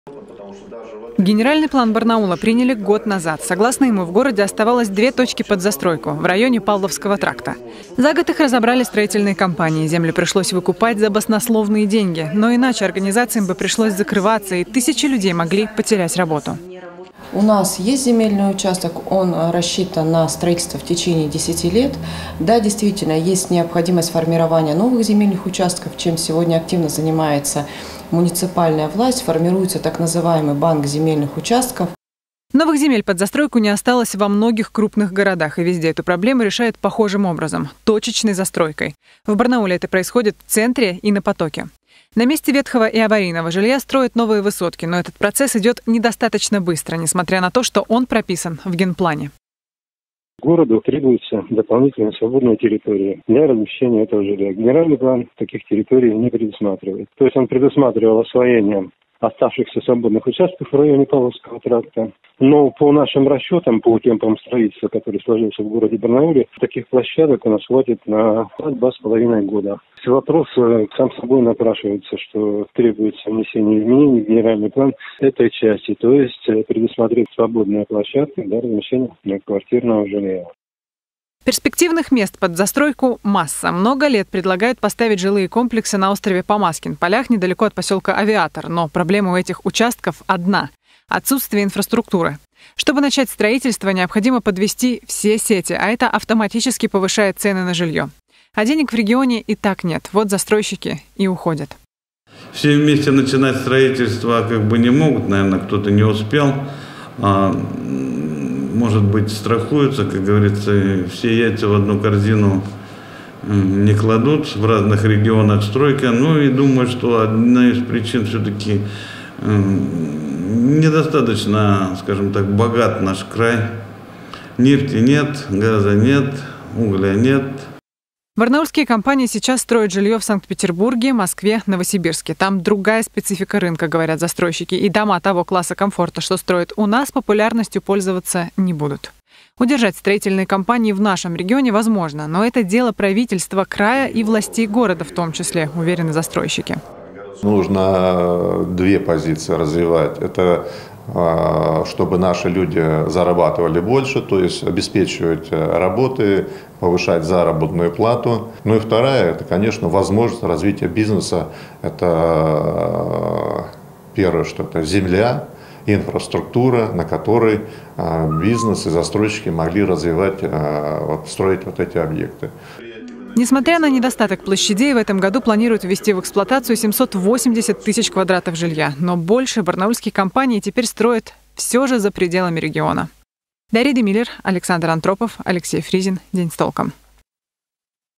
В Барнауле больше нет свободных площадок под застройку | Куда уходят застройщики?
В Барнауле больше нет свободных площадок под застройку. Специалисты убеждены, что именно в этом  причина того, что застройщики уходят из региона. Но только ли в этом вопрос?